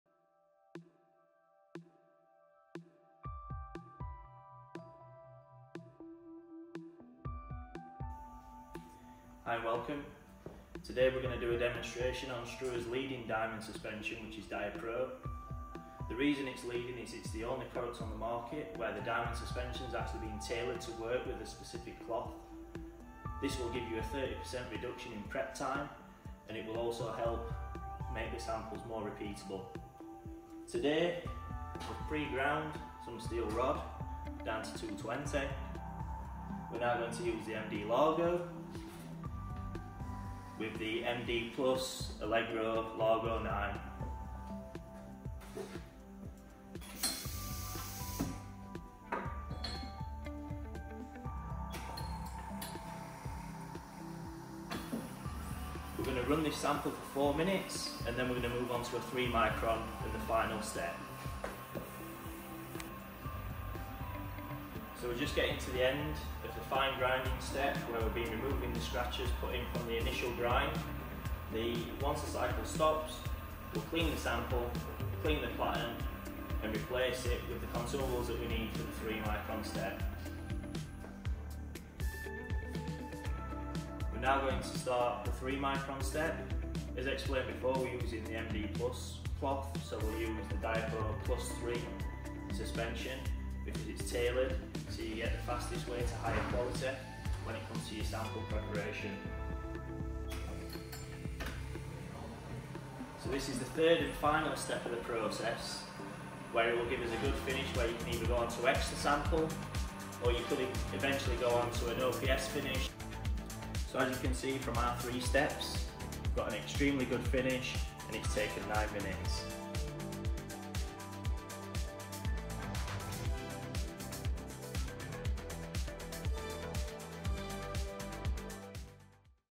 Speed Up Sample Prep with DiaPro Grinding & Polishing
DiaPro saves, on average, 30% preparation time. The diamond suspension is tailored to the cloth and makes your processes quicker throughout every step. Lee Broomhead will show you how to prepare a Steel Rod sample, which has already been through the grinding process to 220 grit.
The first fine grinding step reduces the scratches from the initial grind. Prior to the final step, the sample and the platen will be cleaned and ready for the diamond suspension on the Struers MD - Plus cloth for a 3μm finish. The third and final step of the process will provide a good mirror finish in 9 minutes.